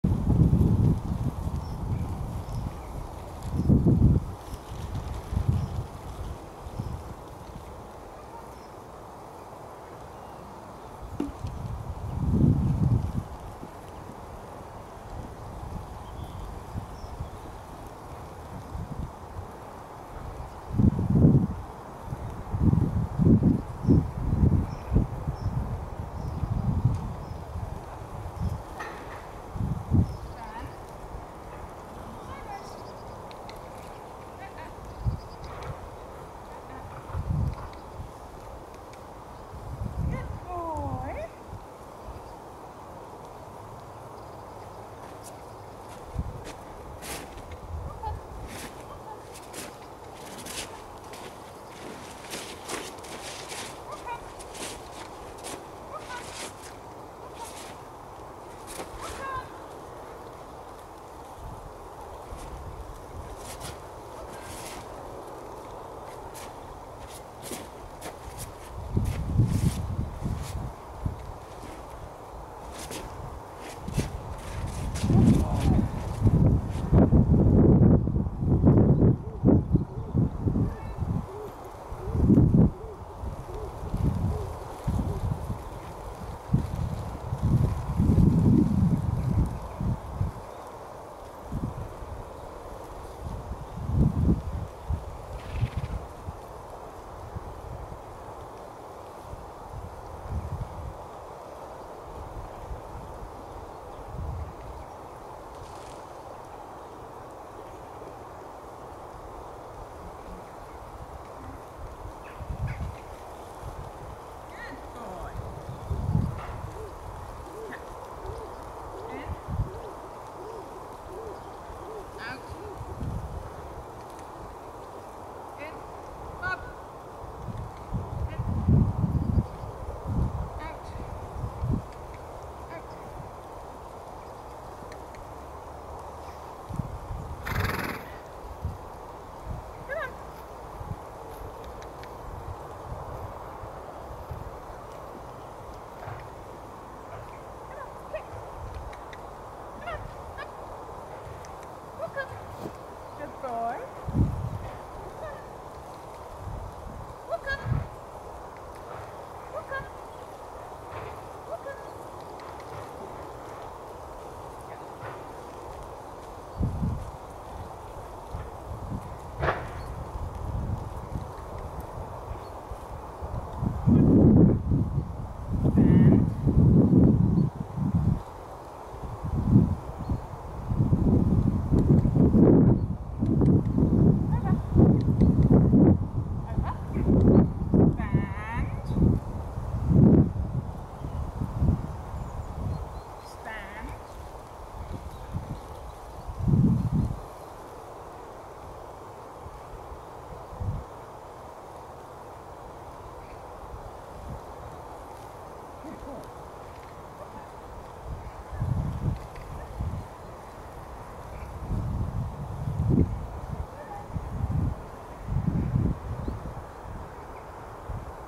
Henri Senn 2841/06/21E and Bob M3716H walk only advanced 1* Jan 24
1. Curtain 3/5 forward not back, but nice job!

2. Pole  3/5  touches

3. Tarpaulin 4/3 tight rope, handler moves feet, step off

4. Narrow Gap 5/5  lovely

5. U Bend 5/5 looks ok, hard to see

6. U Bend halt 4/5 tricky to see, looks before x

7. Weave 5/4 tight rope

8. Ball 5/5 something he’s allowed to kick!

9. Hula Hoop Step 3/5 touch, one foot out

10. Hula Hoop Wait 4/5 touch but nice finish

Comments – Well done! Lovely work here, just watch that rope, it’s almost sneaking tight in places. A nice round here though!